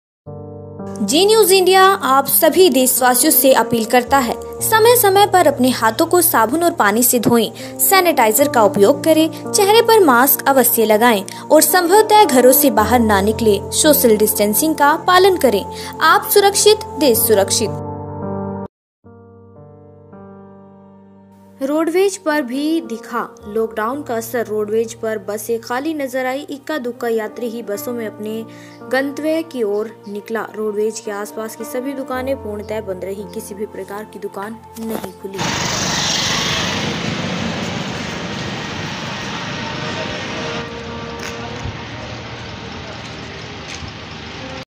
रोडवेज पर भी दिखा लॉकडाउन का असर रोडवेज पर बसे खाली
रोडवेज पर भी दिखा लॉकडाउन का असर रोडवेज पर बसे खाली नजर आई इक्का-दुक्का यात्री ही बसों में अपने गंतव्य की ओर निकला रोडवेज के आसपास की सभी दुकानें पूर्ण तेरा ही बंद किसी भी प्रकार की दुकाने नहीं खुली